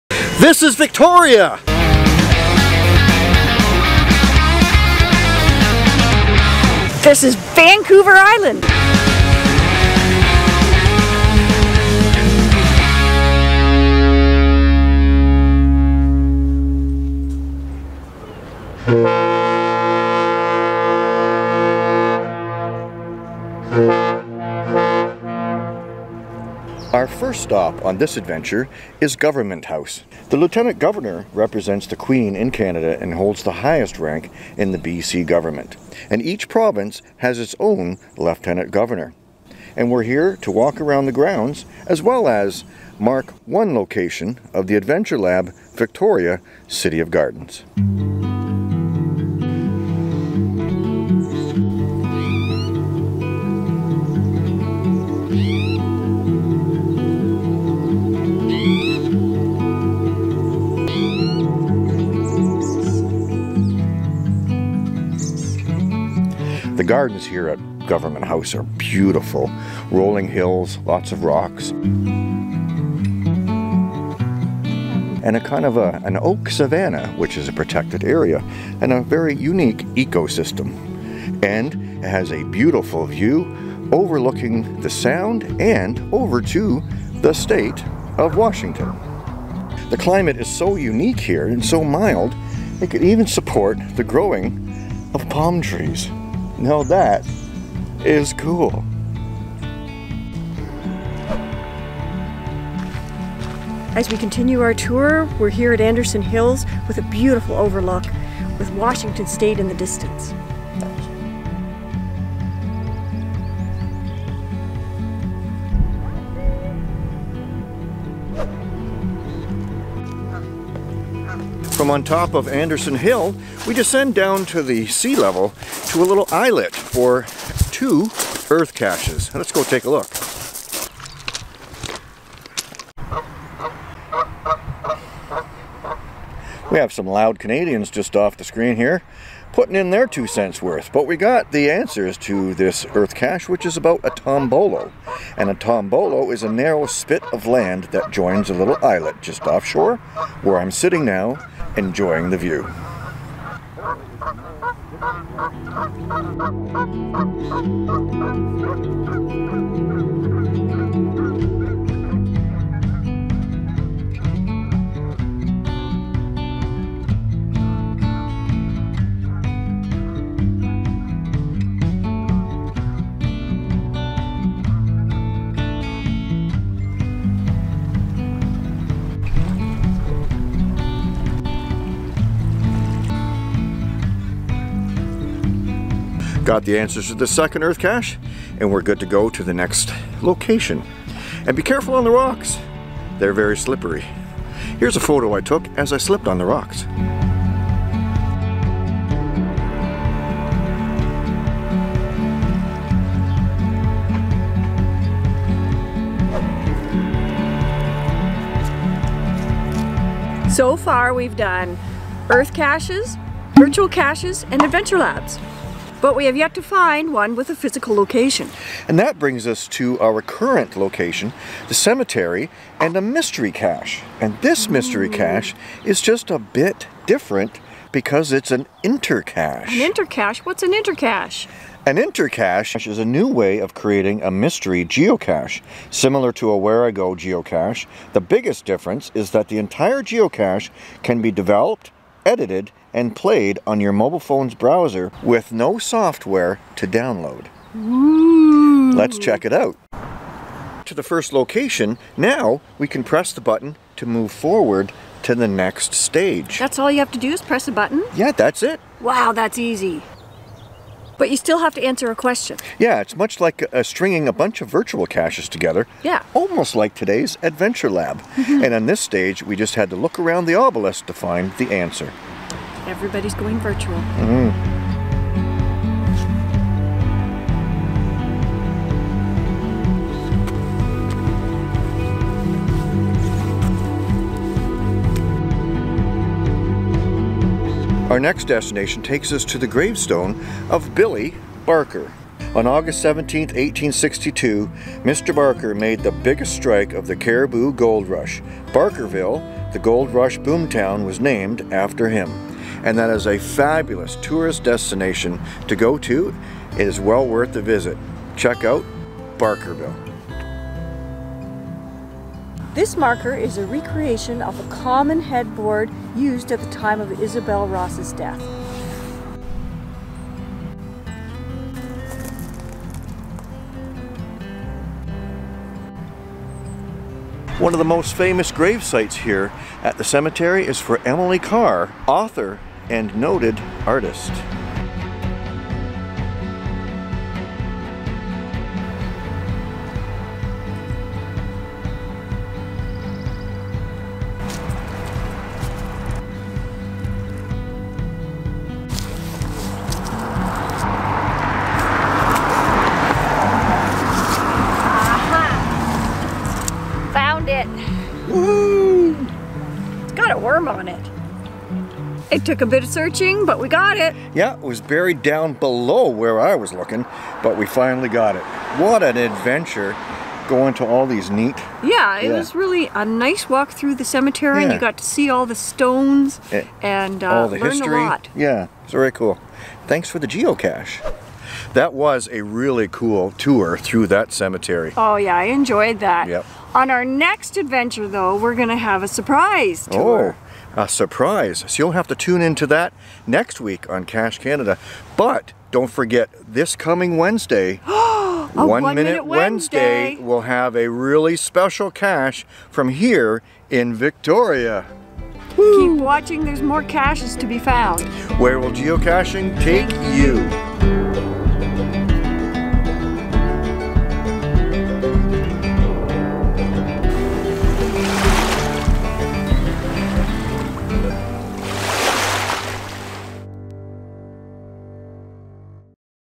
Cache Tour! Geocaching in Victoria and Vancouver Island
Welcome to our first Cache Tour of 2022 as we go geocaching in Victoria, British Columbia, and southern Vancouver Island.